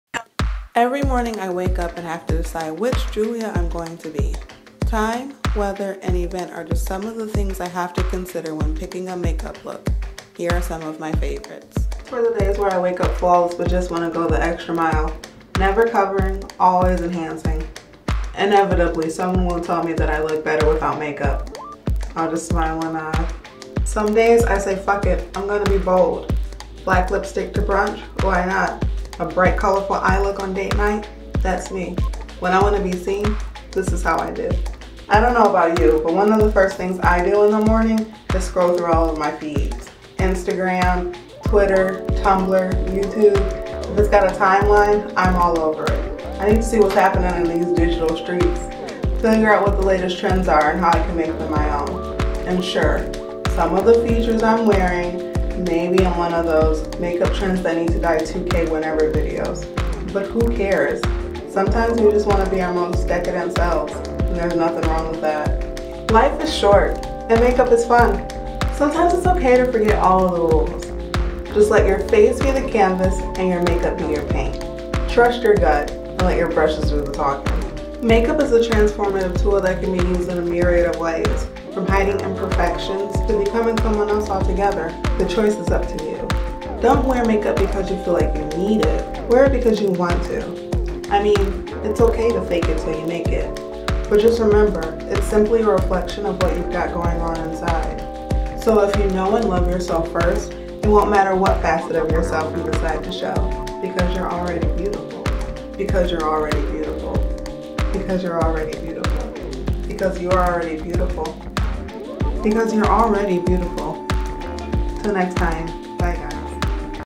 The Power of Makeup | Remixed
I was inspired by NikkiTutorial’s Power of Makeup video, but decided to put my own twist on it. I’ve been wearing makeup since I was about 16 and used to hide behind it. But being able to put on a mask for the world helped me fake my way to confidence until I genuinely gained it. I guess the thing I love the most about makeup is being able to control people’s perception of me. Maybe I’m the “natural” girl that just “needs” mascara and lipgloss, maybe I’m the rebel that will wear a vampy lip all year round, maybe I’m trendy social media devotee trying to perfect her “baddie” eyeliner. Plot twist I’m them all but none of them define me. At the end of the day I’m Julia and Julia is all I’d ever wanted to be.